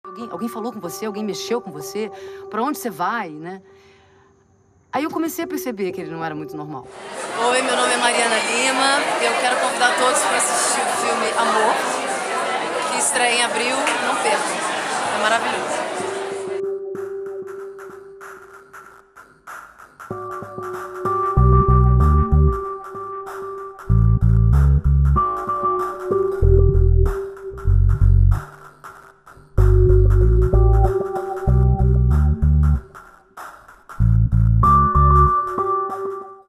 "Amor?" - convite da atriz Mariana Lima
Mariana Lima convida para "Amor?", novo filme de João Jardim.